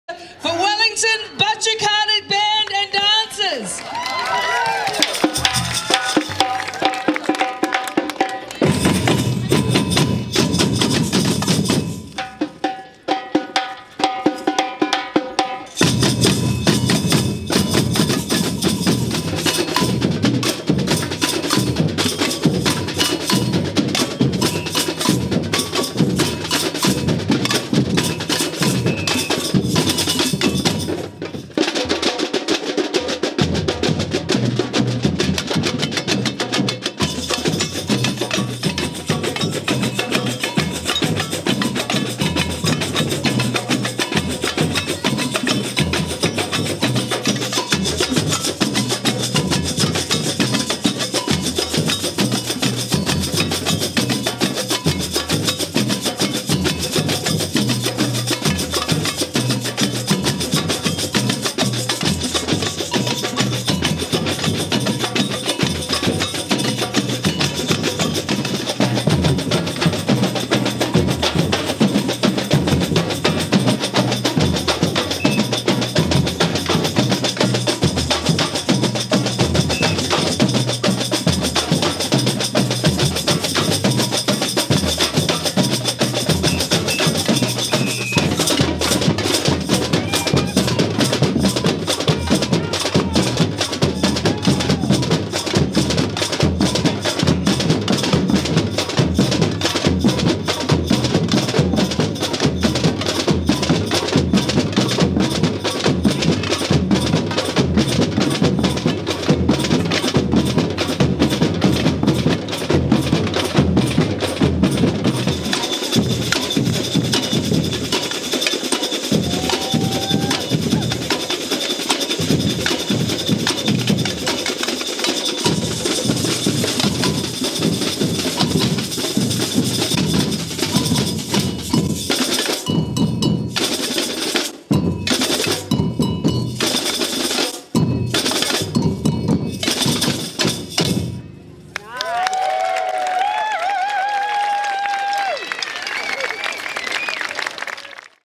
Wellington Batucada at Coastlands Carnival, November 2019
Wellington Batucada drummers and dancers perform at the 50th Birthday Celebration for Coastlands Mall in Paraparaumu, 9 November 2019.

Video by Tom S Etuata.